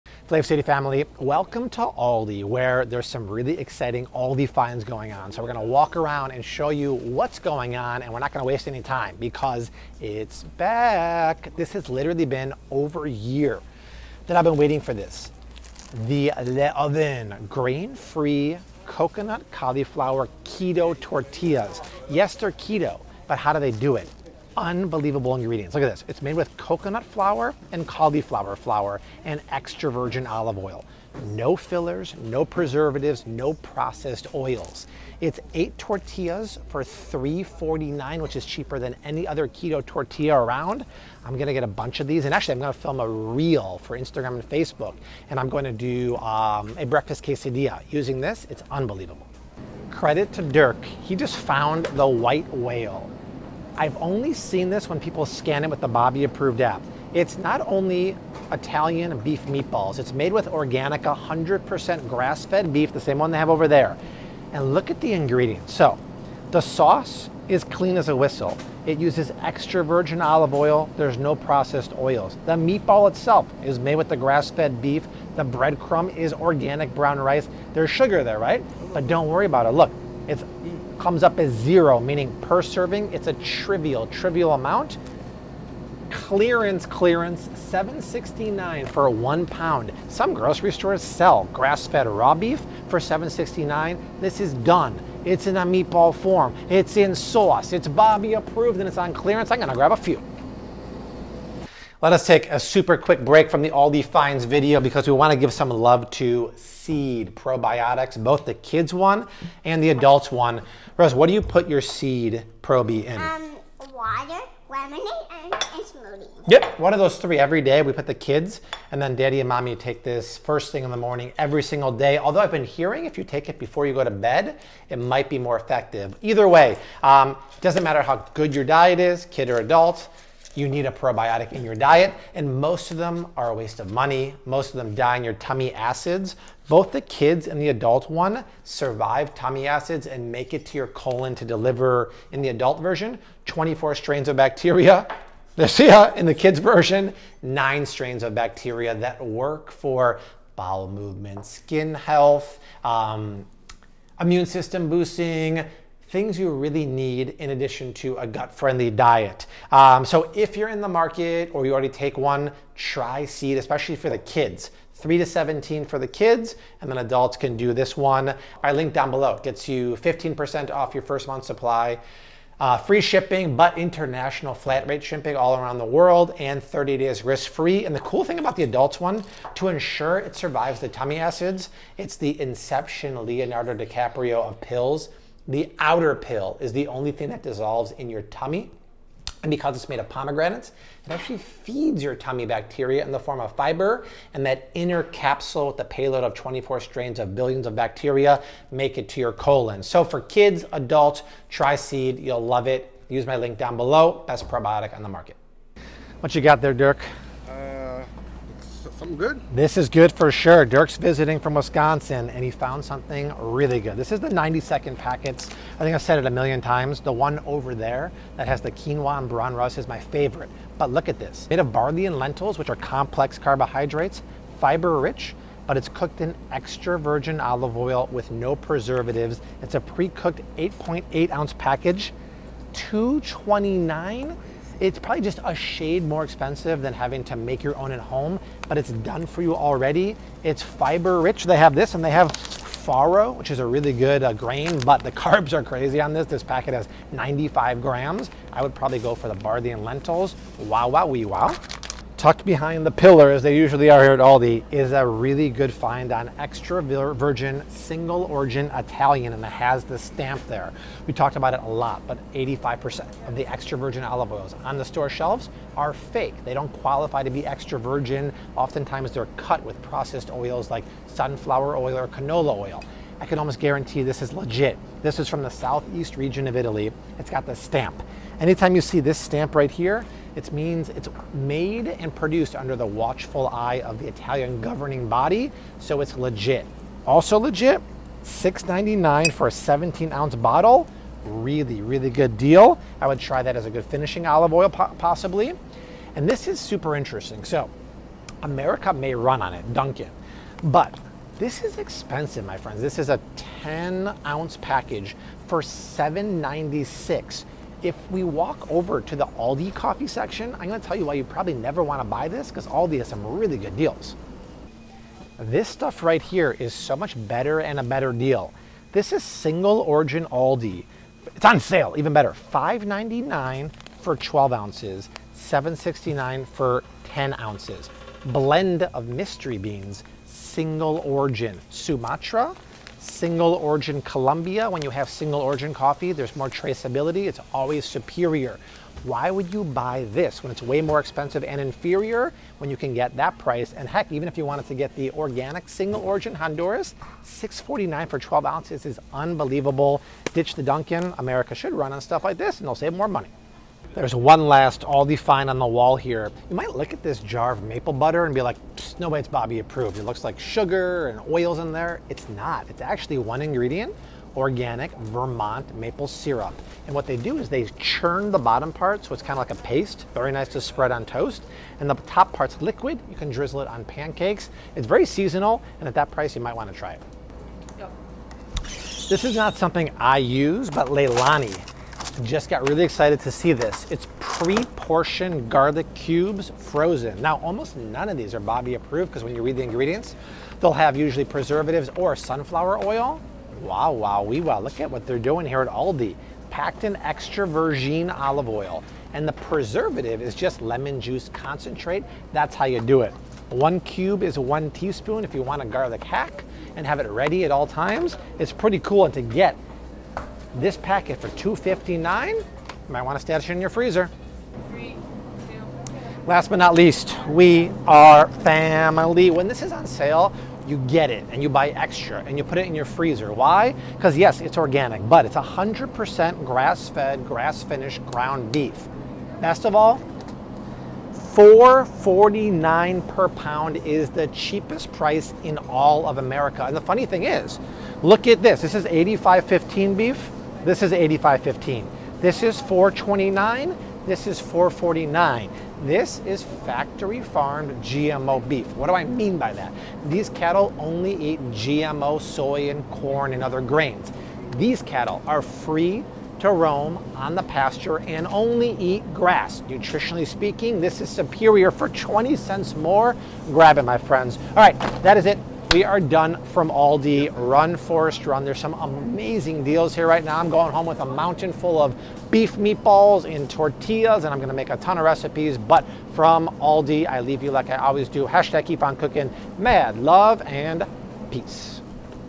Top 10 ALDI Finds RIGHT NOW!
There are some great ALDI finds happening right now, including one of the best keto tortillas! :et's go shopping at ALDI.